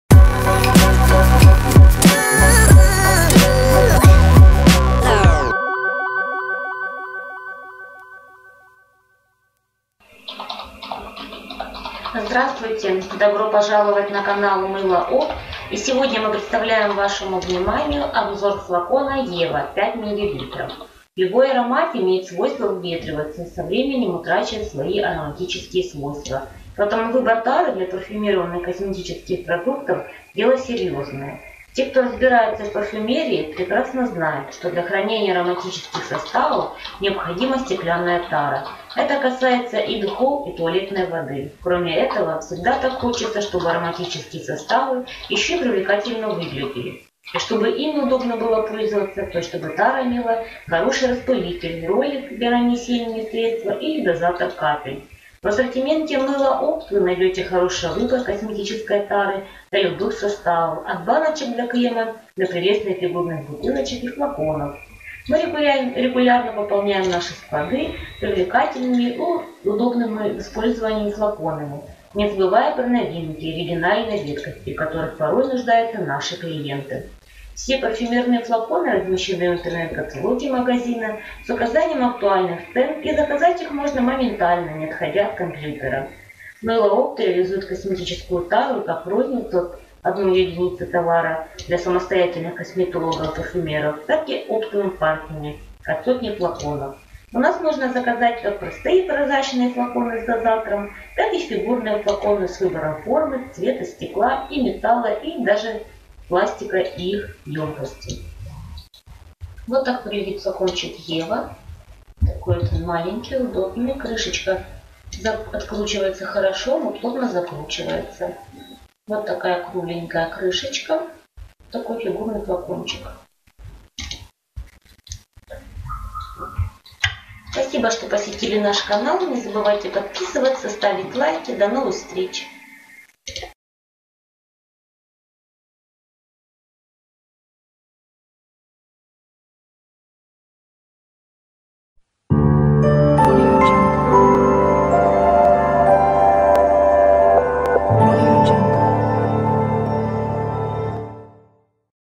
Флакон Ева, 5 мл // Тара для парфюмерии // Духи делаем дома
Видео обзор Флакон Ева, 5 мл от магазина Мыло-опт